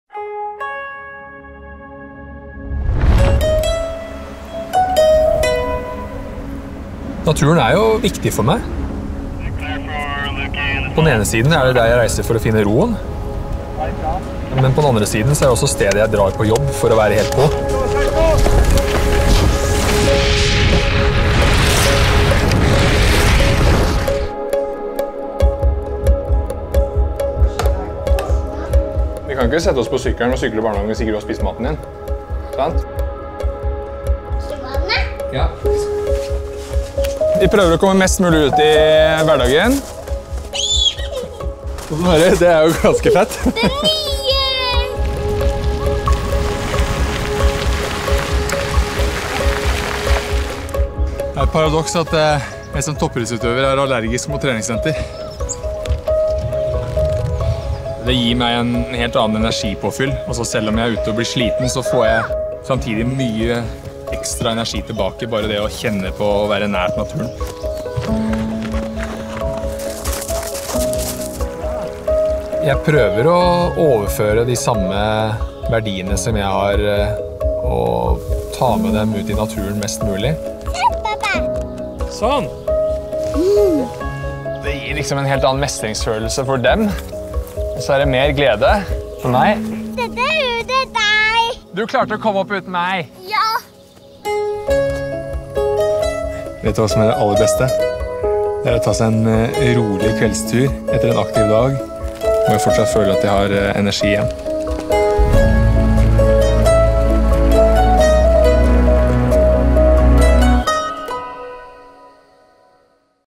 Urkraft - næring til nye opplevelser - Leif Nestvold-Haugen
Møt Urkraft ambassadør Leif Kristian Nestvold-Haugen.
Urkraft er en serie havrebaserte produkter fra Møllerens.
Produktene gir deg langvarig energi og næring til nye opplevelser.
Urkraft oppfordrer deg til å bruke den gode næringen og energien fra havren til tråkke opp stiene du har drømt om – slik at du kan oppleve mer.